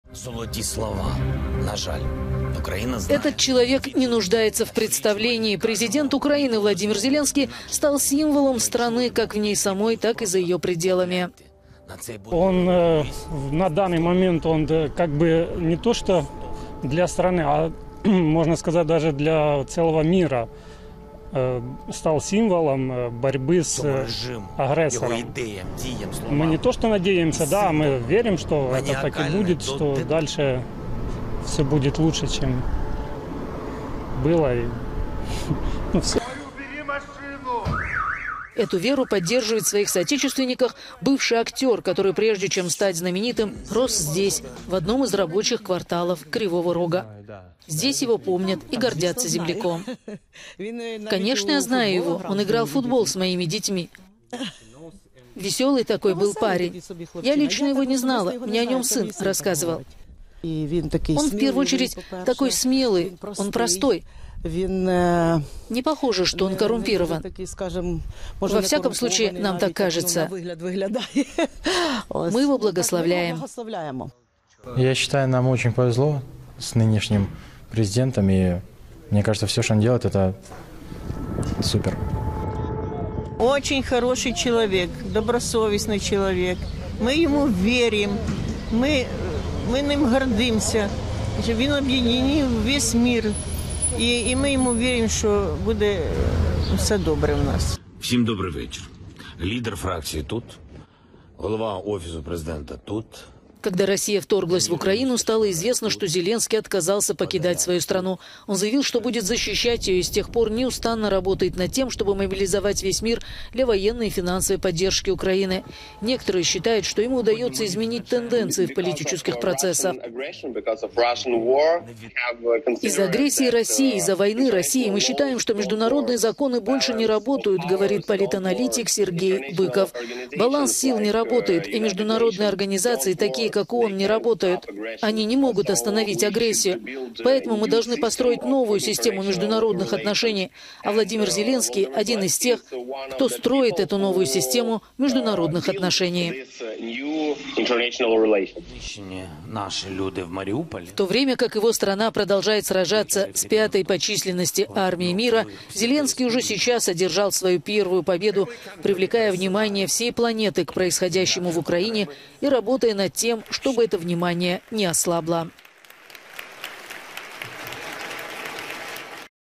Украинцы о Зеленском: "Мы его благославляем"
Украинцы считают, что им повезло с президентом - Владимир Зеленский стал символом борьбы своей страны далеко за её пределами.